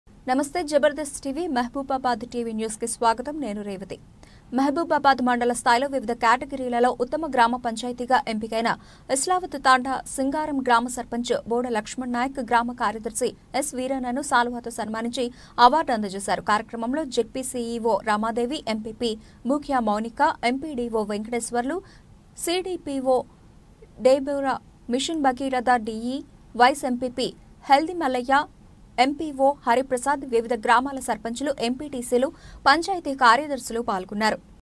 జబర్దస్త్ టీవీ //MAHABUBABAD TV //ఉత్తమ గ్రామ పంచాయతీగా ఇస్లావత్ తండా సింగారం
మహబూబాద్ మండల స్థాయిలొ వివిధ కేటగిరీలలో ఉత్తమ గ్రామ పంచాయతీగా ఎంపికైన ఇస్లావత్ తండా సింగారం గ్రామ సర్పంచ్ బోడ లక్ష్మణ్ నాయక్, గ్రామ కార్యదర్శి ఎస్ వీరన్న ను శాలువాతో సన్మానించి, ,అవార్డు అంద చేశారు.
 కార్యక్రమంలో జడ్పీ సీఈవో రమాదేవి, ఎంపీపీ భుఖ్య మౌనిక,
 ఎంపీడీవో వెంకటేశ్వర్లు, సిడిపిఓ  డేబొర, మిషన్ భగీరథ డి ఈ, వైస్ ఎంపీపీ హెల్ది మల్లయ్య, mpo హరిప్రసాద్, వివిధ గ్రామాల సర్పంచులు, ఎంపీటీసీలు, పంచాయతీ కార్యదర్శులు పాల్గొన్నారు.